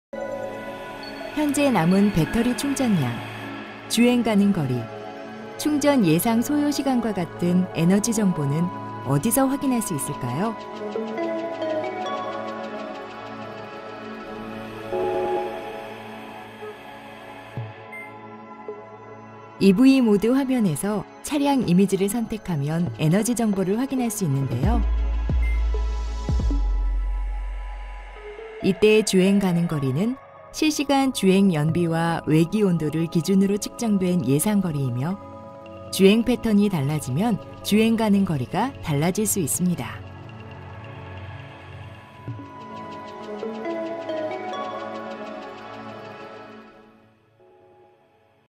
취급설명서 아이오닉 5 에너지 정보 확인(광고 48초)
하나부터 열까지 모든 게 생소한 전기차 아이오닉, 어디서부터 시작해야할까요?🤔
회생 제동부터 충전 정보까지, 전~부 알려드릴게요!🚙

아이오닉을 보다 쉽고 편리하게 경험하는 방법은 아이오닉 디지털 오너스 매뉴얼에서도 확인하실 수 있습니다.